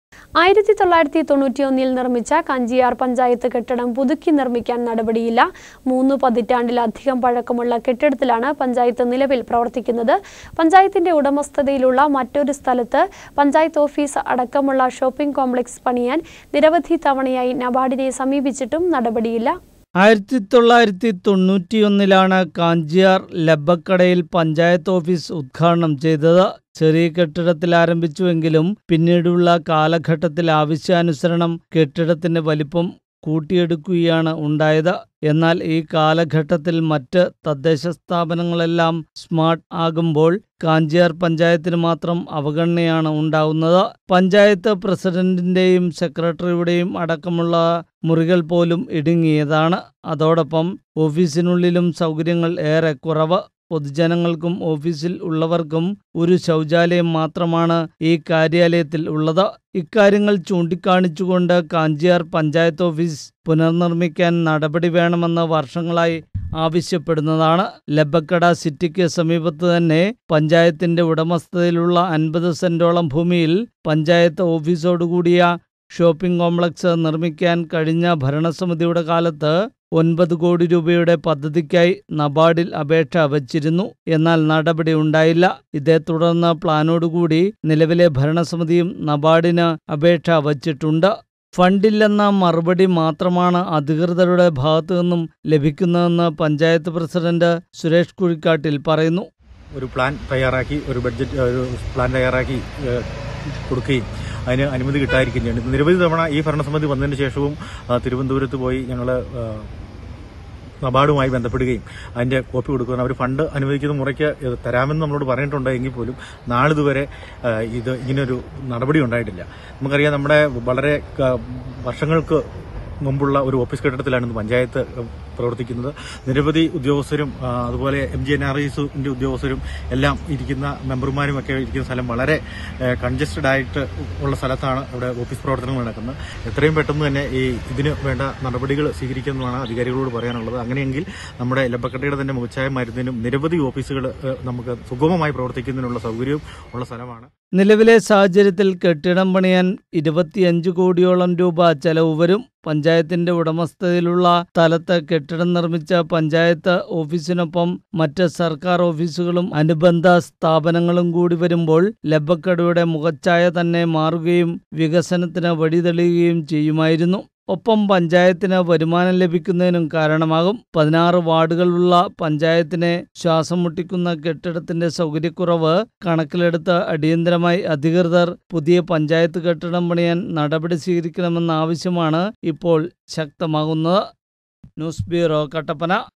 കാഞ്ചിയാര്‍ പഞ്ചായത്ത് കെട്ടിടം പുതുക്കിനിര്‍മ്മിക്കാന്‍ നടപടിയില്ല
1991-ല്‍ നിര്‍മ്മിച്ച കാഞ്ചിയാര്‍ പഞ്ചായത്ത് കെട്ടിടം പുതുക്കിനിര്‍മ്മിക്കാന്‍ നടപടിയില്ല. മൂന്നു പതിറ്റാണ്ടിലധികം പഴക്കമുള്ള കെട്ടിടത്തിലാണ് പഞ്ചായത്ത് നിലവില്‍ പ്രവര്‍ത്തിക്കുന്നത്. പഞ്ചായത്തിന്റെ ഉടമസ്ഥതയിലുള്ള മറ്റൊരു സ്ഥലത്ത് പഞ്ചായത്ത് ഓഫീസ് അടക്കമുള്ള ഷോപ്പിംഗ് കോംപ്ലക്‌സ് പണിയാന്‍ നിരവധി തവണയായി നബാര്‍ഡിനെ സമീപിച്ചിട്ടും നടപടിയില്ല.